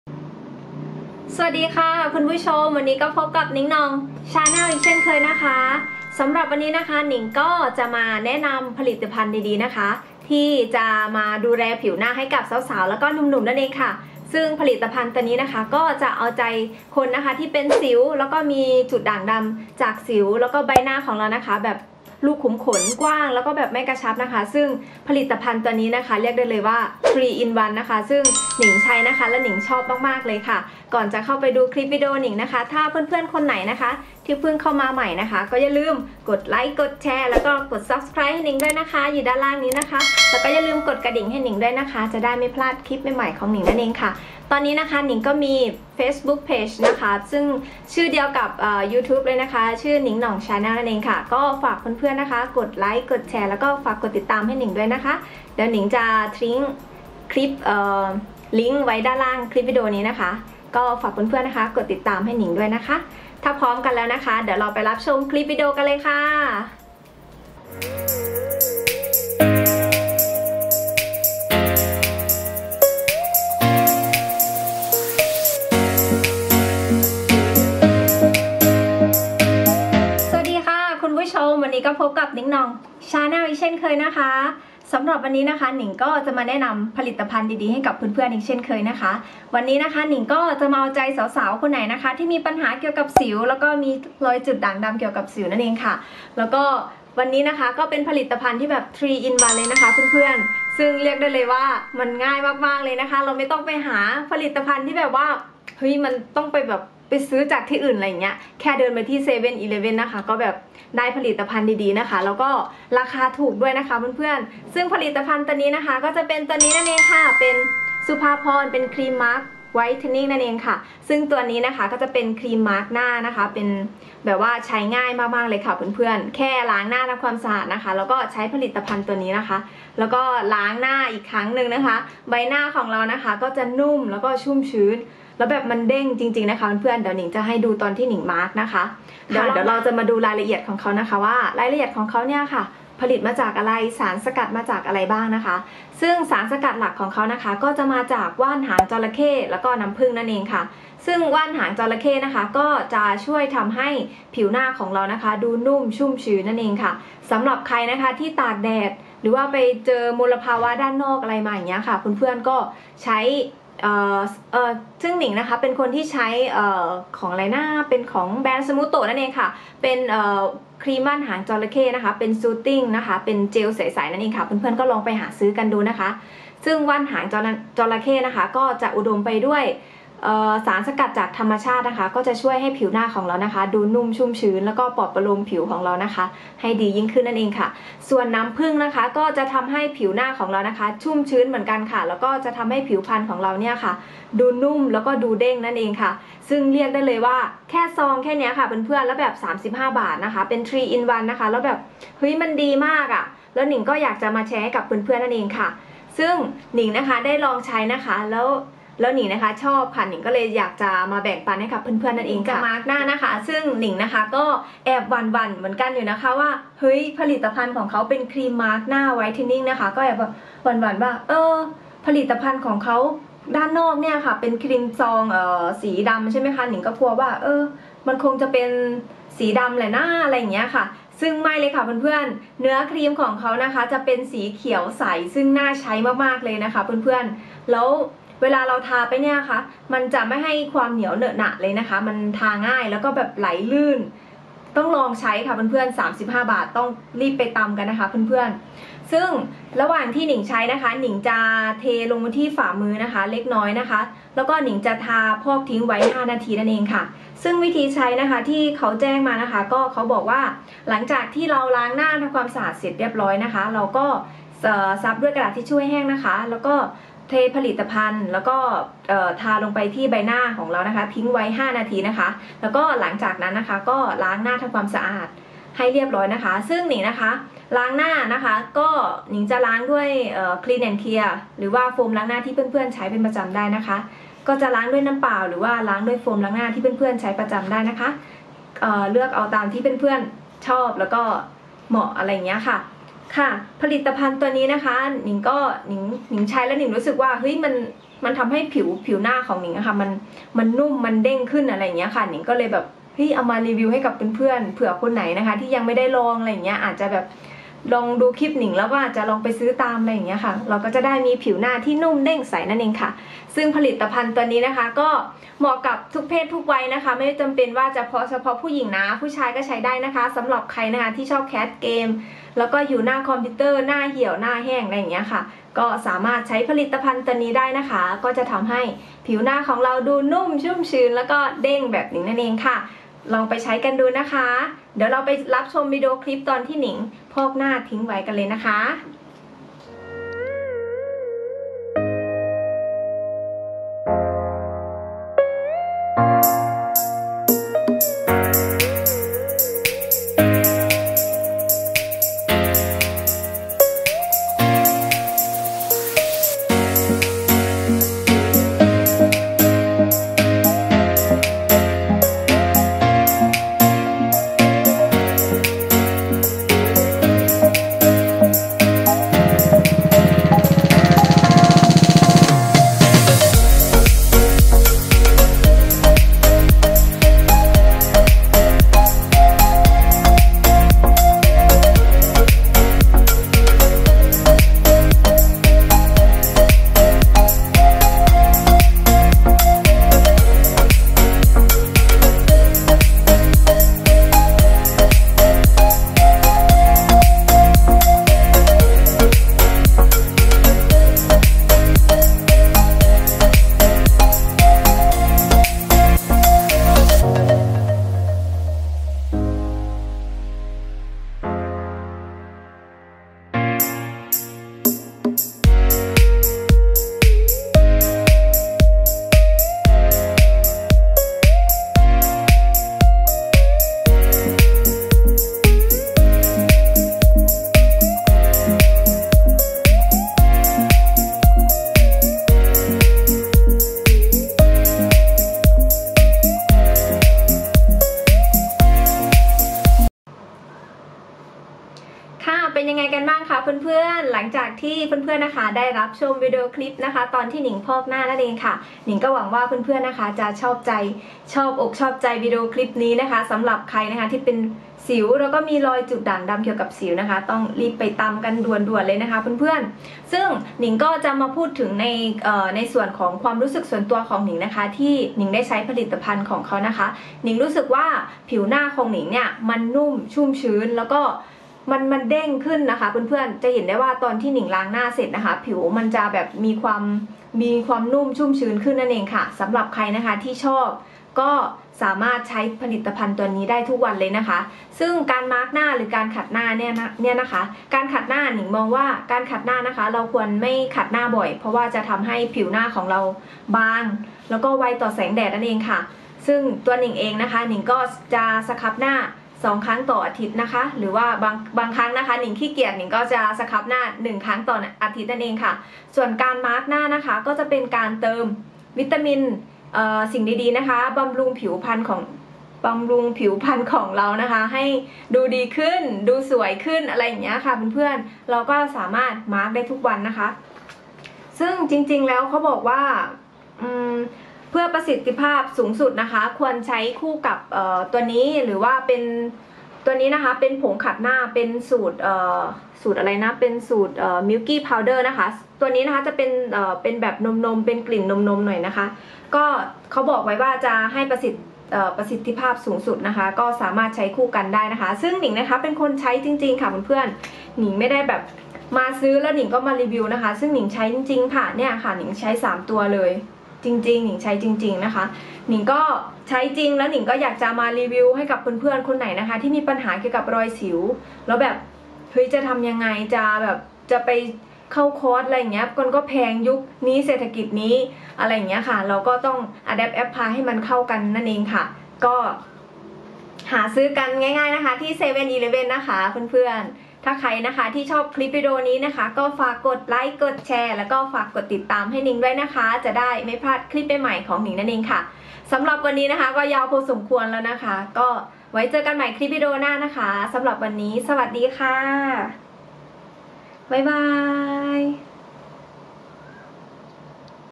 รีวิวครีมมาส์คหน้าไวท์เทนนิ่งช่วยลดรอยสิว💖แบบ 3 in 1 By Supaporn Cream Mask Whitening🌿
"ครีมมาส์คหน้า ไวท์เทนนิ่ง (สารสกัดว่านหางจระเข้ + น้ำผึ้ง)" การดูแลผิวหน้าจากสิว และรอยด่างดำจากการบีบสิวเพื่อผิวหน้าที่แลดูขาวกระจ่างใส

วิธีดูเเลผิวหน้าด้วยมาส์กโคลนไวท์เทนนิ่ง ใช้เเล้วดีมาก หน้ากระจ่างใส ถูกเเละดี
ช่ อ ง ท า ง ก า ร ติ ด ต่ อ